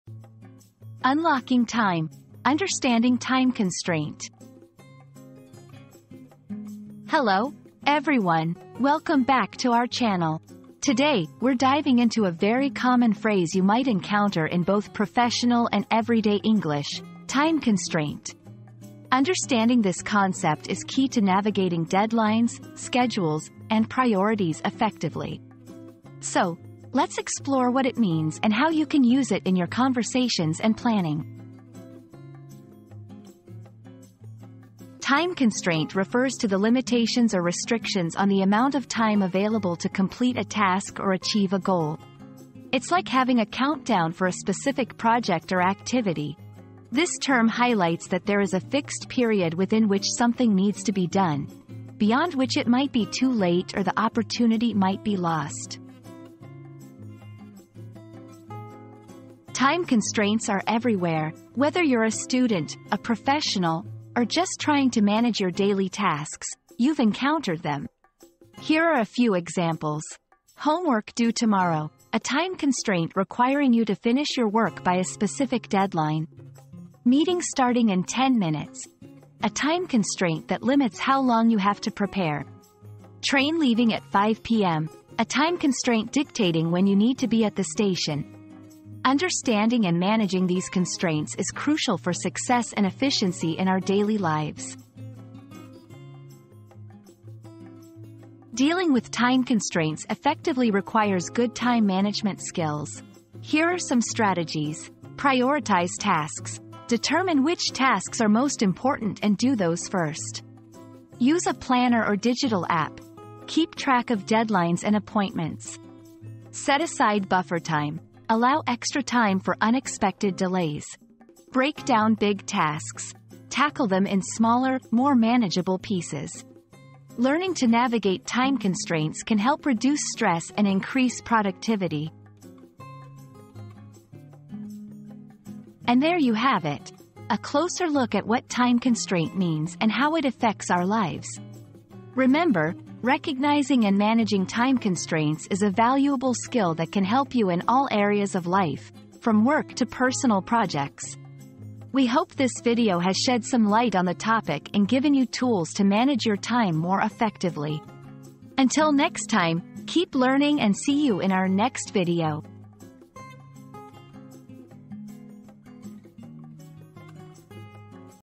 Unlocking Time: Understanding Time Constraint
00:00 • Introduction - Unlocking Time: Understanding Time Constraint
00:34 • What Does "Time Constraint" Mean?
00:59 • Examples of Time Constraints in Everyday Life
01:37 • How to Deal with Time Constraints